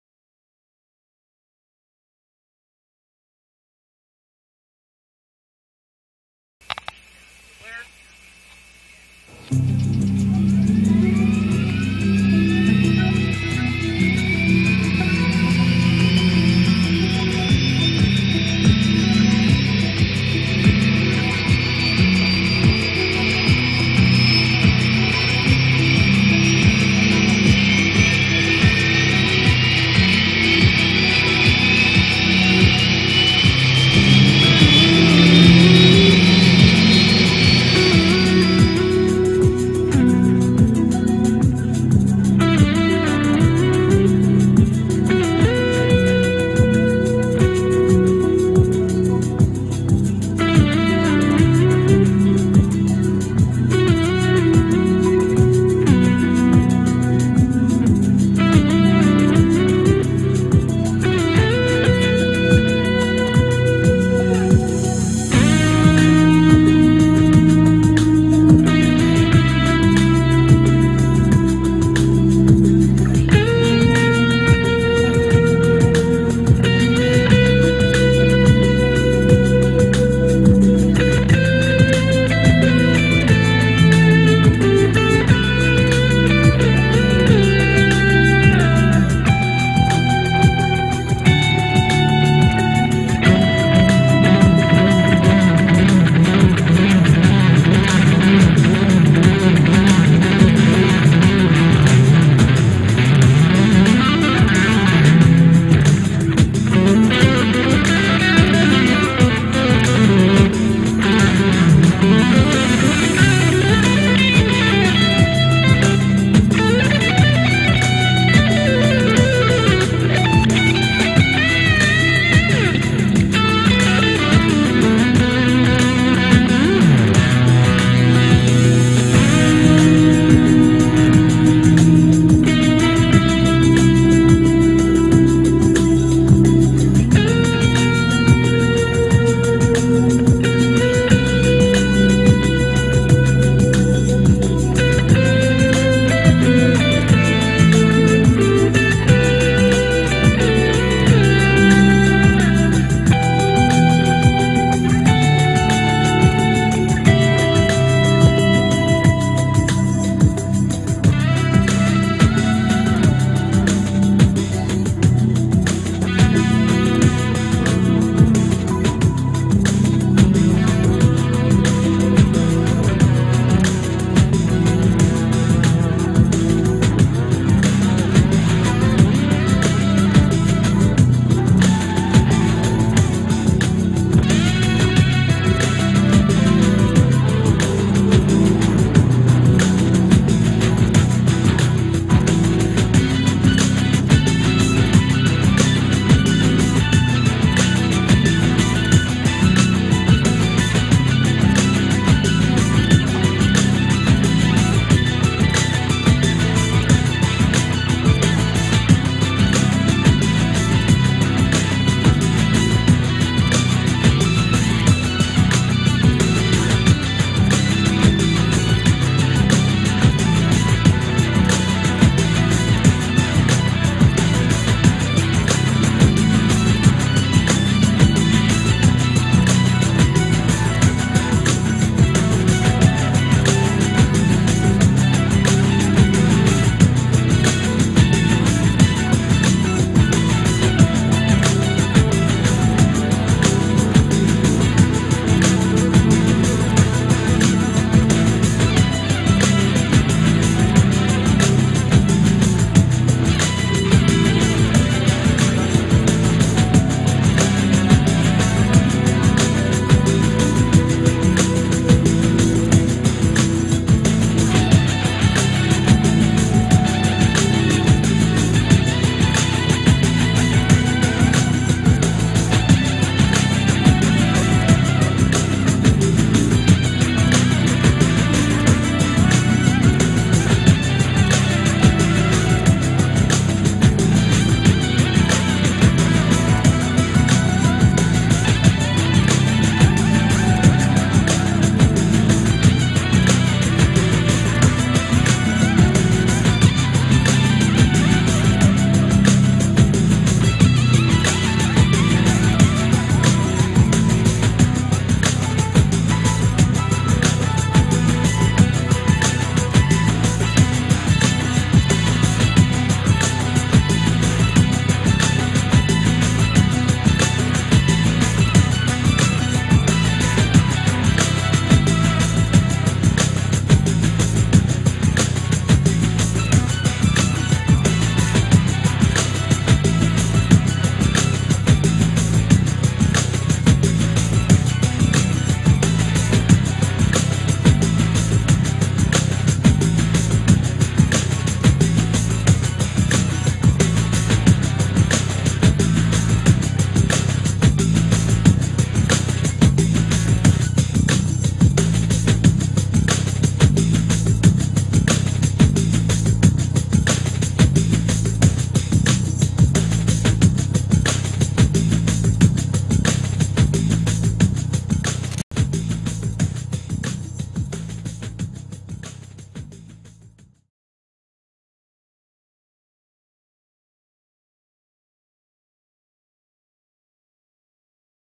Wings over Memphis
Testing the gyro mount cam over downtown Memphis and Mississippi river. Pickle T.V.